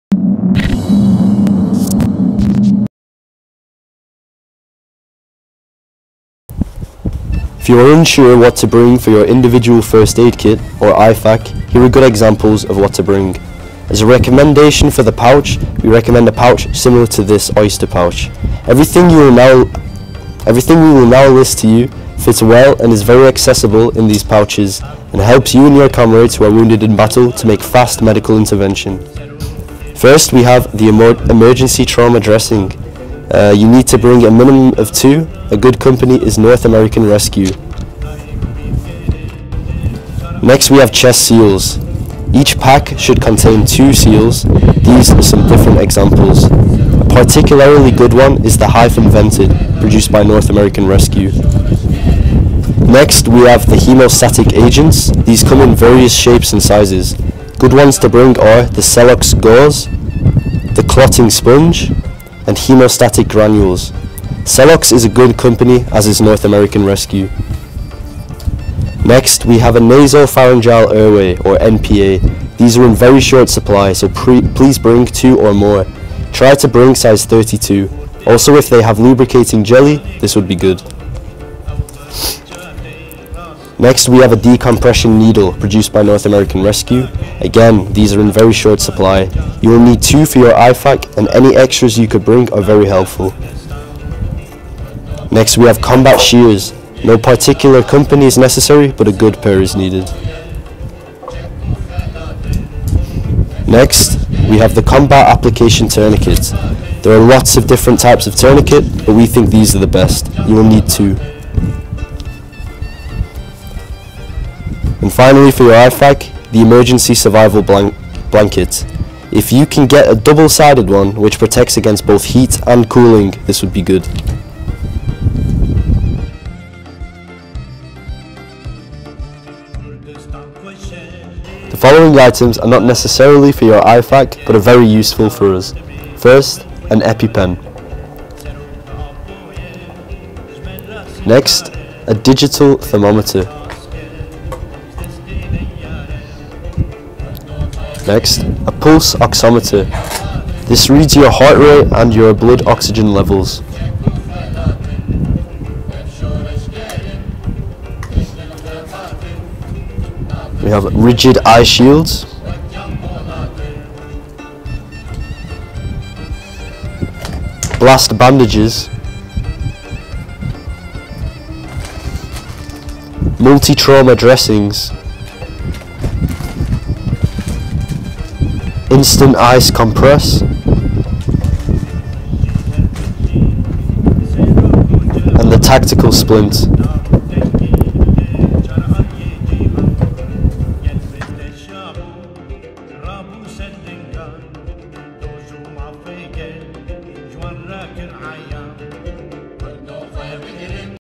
YPG International IFAK - Medical Equipment To Bring
This video contains a short guide for medical equipment that we think should go in every Individual First Aid Kit (IFAK) here in our unit. Everything what is not in the video is easy to provide here.
Here is the list:
First of all the pouch itself to fit all the equipment in (Oyster Pouch)

- Emergency Trauma Dressing 2+
- Chest Seals 2+ (best ones are Hyfin Vented)
- Haemostatic Agents (Celox Gauze, Clotting Sponge, Haemostatic Gauze)
- Nasopharyngeal Airway (NPA) 2+ / Size 32
- Decompression Needle 2+
- Combat Shears
- Combat Application Tourniquet (CAT) 2+
- Emergency Survival Blanket (2 sided)

Not in the video but also helpful:
- Permanent marker (felt tip)

Additional medical supplies:
- Epi-pen
- Digital Thermometer
- Pulse Oximeter
- Rigid Eye-Shield 2+
- Blast Bandages
- Multi-trauma Dressing
- Instant Ice Compress
- Tactical Splint

Reminder: Before bringing any of this equipment please check your country's export laws for medical equipment!